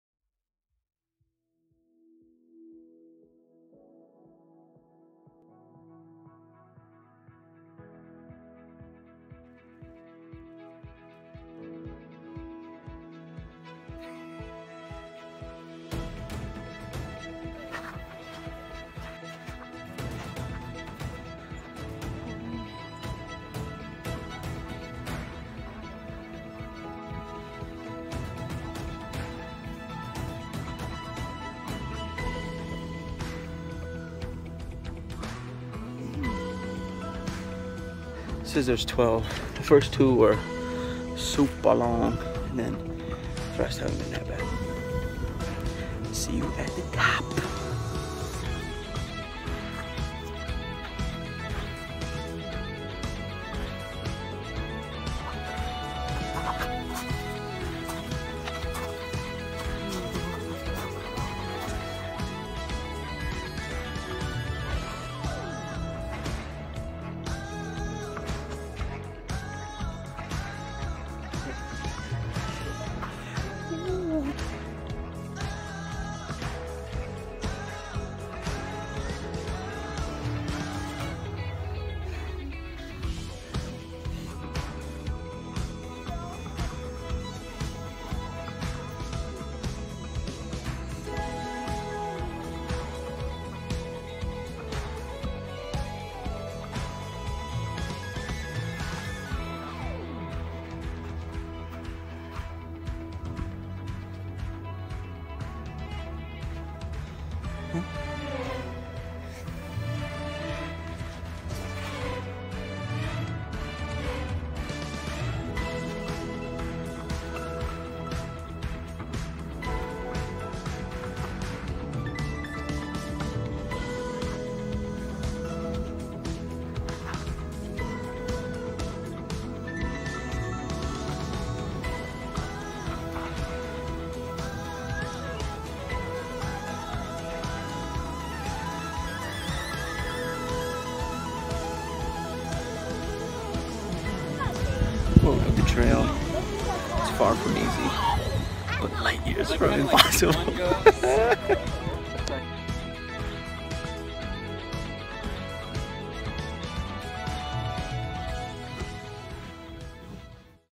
We hiked the Y!
Ulu got up one Saturday morning and off the cuff just said today we are going to do it. It had been 8-10 years since either of us had done it back at SOAR. Heres a little video about the journey